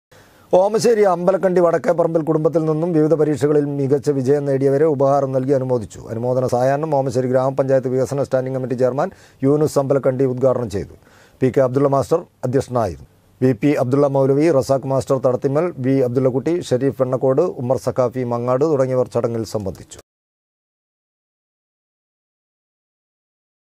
വിവിധ പരീക്ഷകളിൽ മികച്ച വിജയം നേടിയവരെ ഉപഹാരം നൽകി അനുമോദിച്ചു
ഓമശ്ശേരി അമ്പലക്കണ്ടി വടക്കേപറമ്പിൽ  കുടുബത്തിൽ നിന്നും വിവിധ പരീക്ഷകളിൽ മികച്ച വിജയം നേടിയവരെ ഉപഹാരം നൽകി അനുമോദിച്ചു.അനുമോദന സായാഹ്നം ഓമശ്ശേരി ഗ്രാമപഞ്ചായത്ത്‌ വികസന സ്റ്റാന്റിംഗ്‌ കമ്മിറ്റി ചെയർമാൻ യൂനുസ്‌ അമ്പലക്കണ്ടി ഉൽഘാടനം ചെയ്തു